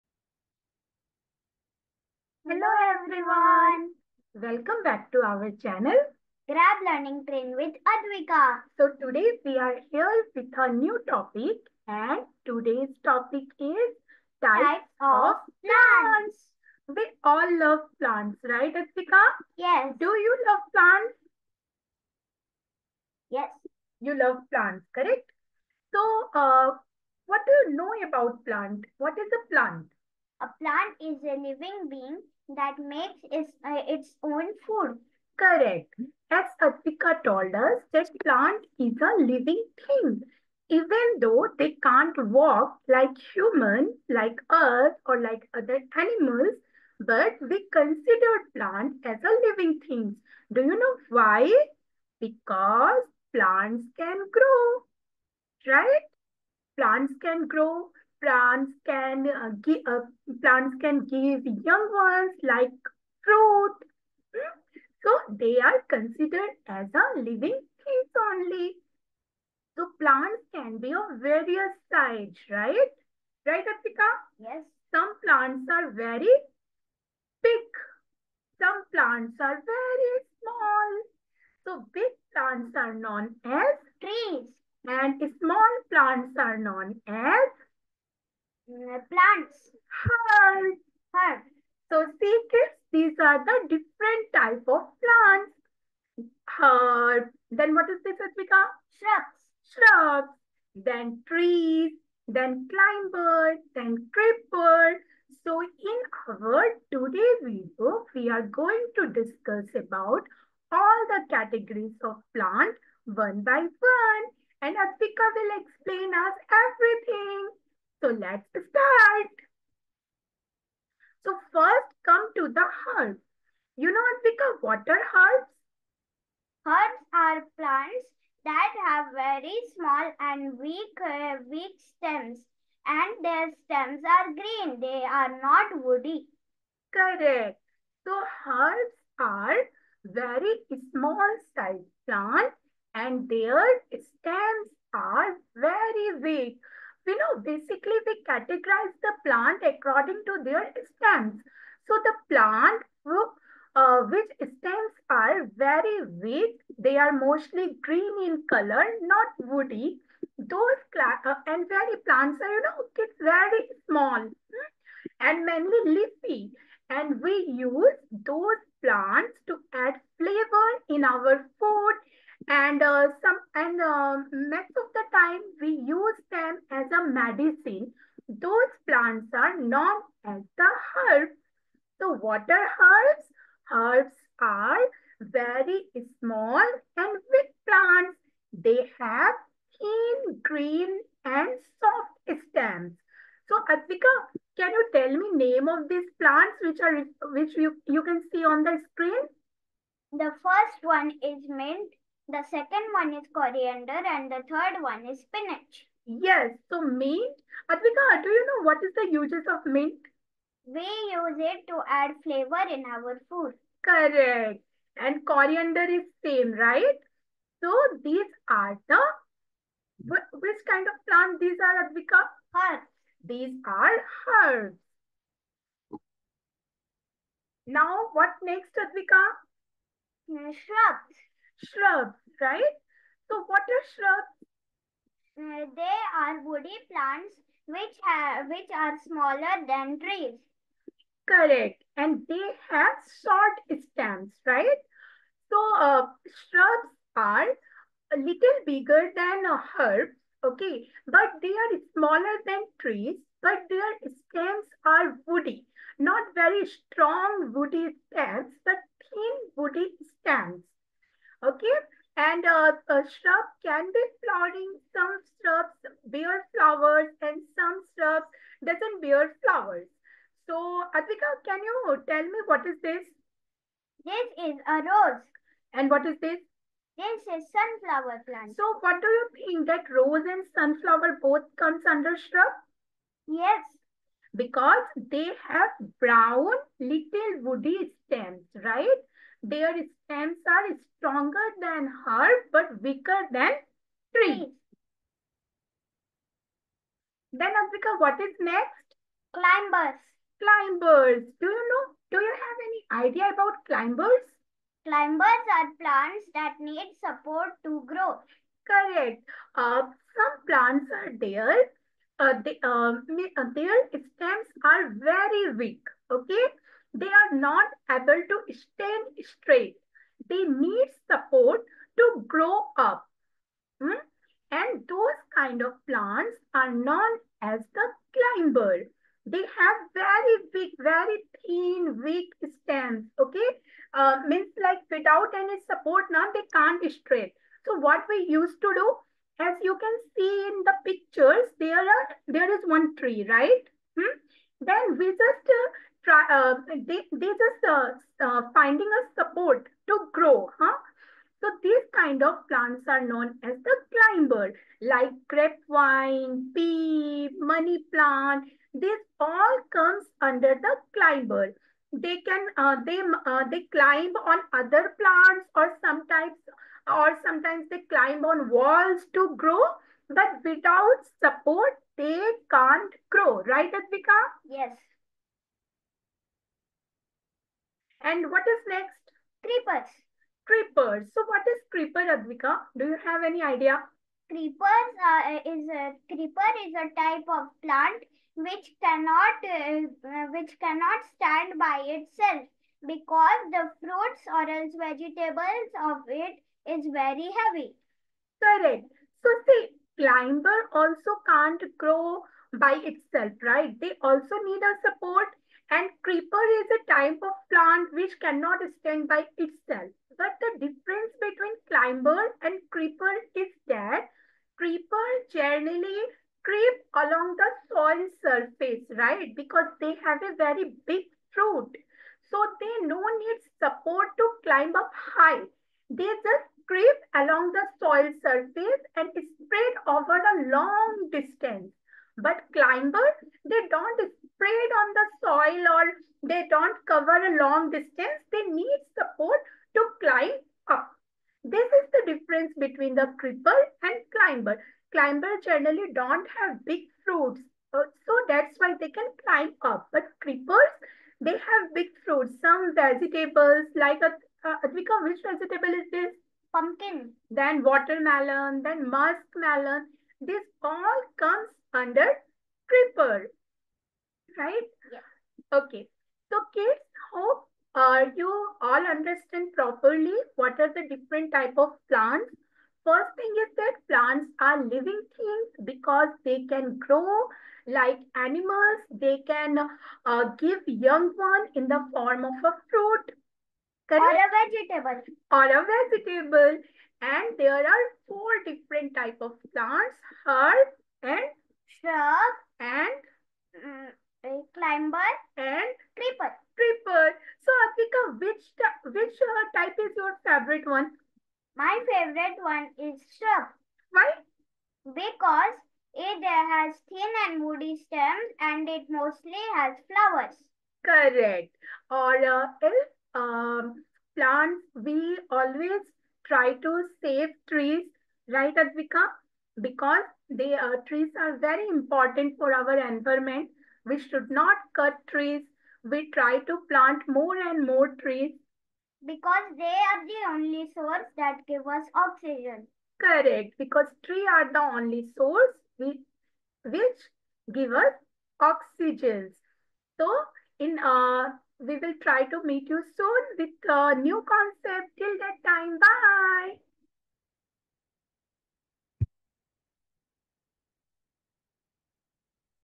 Types of plants Class 1 & 2| herbs | Climbers | Shrubs | Creepers |Trees | EVS |Olympiad
Hello everyone ,This video mainly focuses on the concepts which helps students to know about different types of plants.
Plants are living things and very important for us. In this video we will learn all about the different types of plants with examples.
This video will be very helpful to those students preparing for exams like Olympiad.

Video - Human Body Organs| External & Internal body parts and their functions| OUR BODY-CLASS 2| EVS